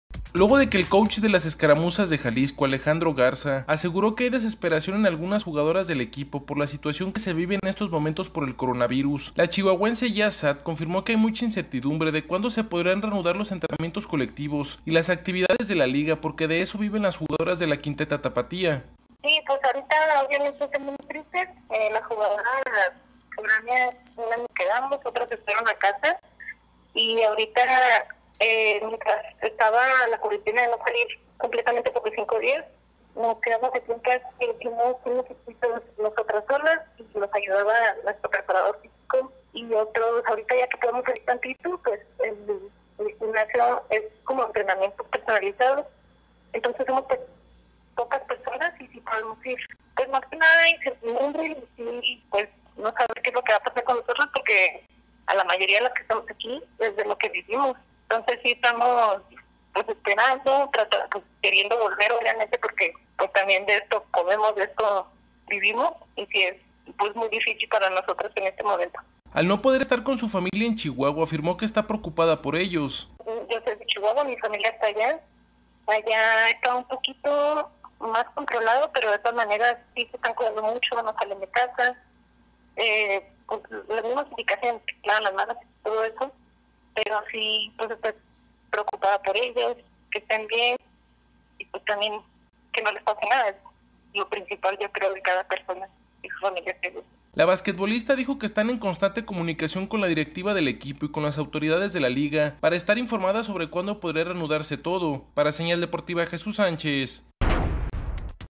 Hay incertidumbre en las jugadoras de Escaramuzas de Jalisco de cuándo regresará la actividad
Hay incertidumbre en las jugadoras de Escaramuzas de Jalisco  de cuándo regresará la actividad de la liga Profesional de baloncesto.